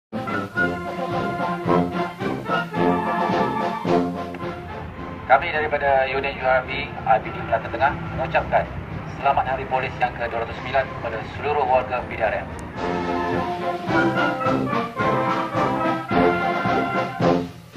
UCAPAN PENGHARGAAN SEMPENA HARI POLIS (37)
Ucapan Selamat Ulang Tahun oleh Unit Rondaan Bermotor (URB) dari IPD Melaka Tengah kepada seluruh warga Polis Diraja Malaysia yang bakal menyambut Hari Polis Ke-209.

Terima kasih di atas kiriman video ini!